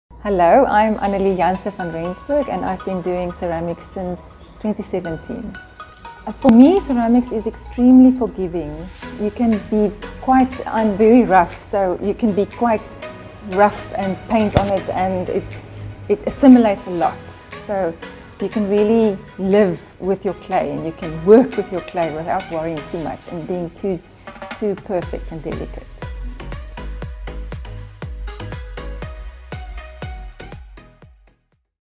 Annelie Janse van Rensburg
Introducing Annelie Jansen van Rensburg as she gives insight to her experience as a ceramic artist and working with clay as a medium.